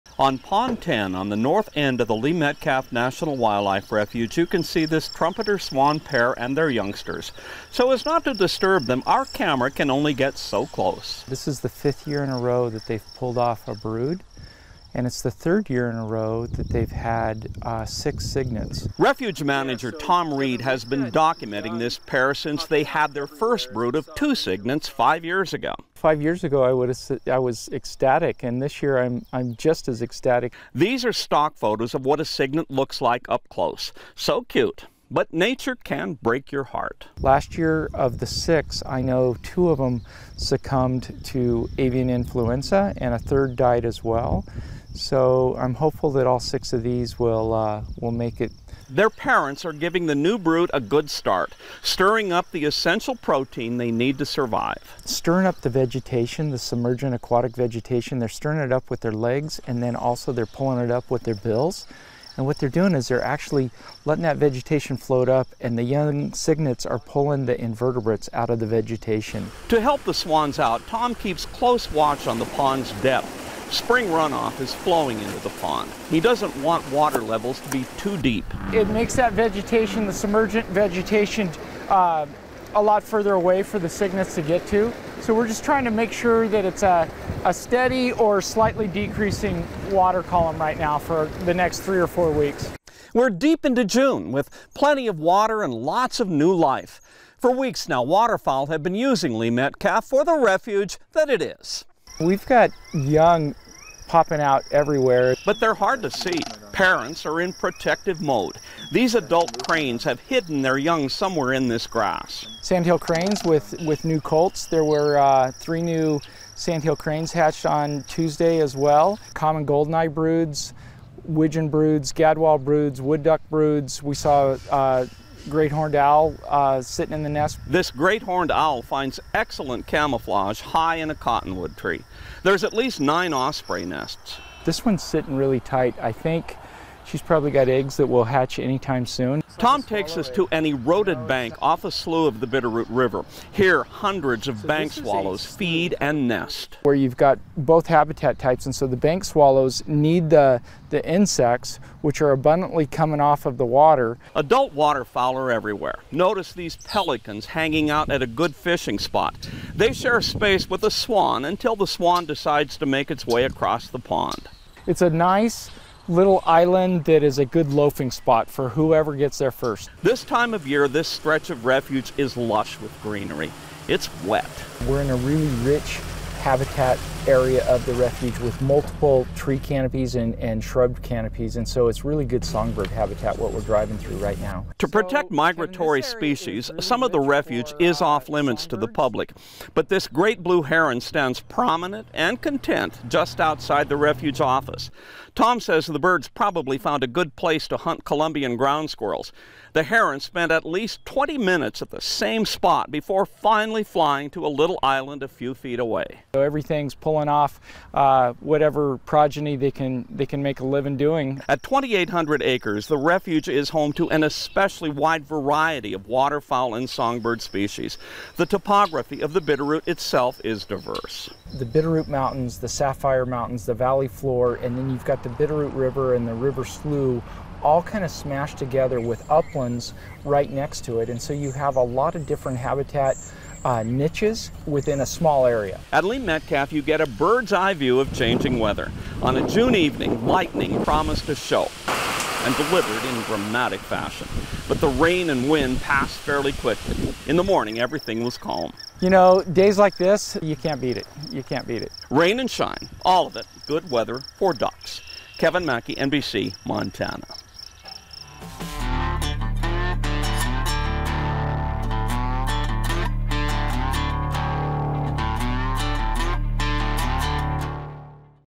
Season welcomes new youngsters to Lee Metcalf National Wildlife Refuge
For several years now, a Trumpeter Swan pair has successfully raised clutches at Lee Metcalf National Wildlife Refuge.
Before that, Trumpeter Swans were only known to migrate through Metcalf.
So, it's a big deal the pair is once again nurturing young at the refuge in 2023.
Refuge manager Tom Reed took NBC Montana to see the family.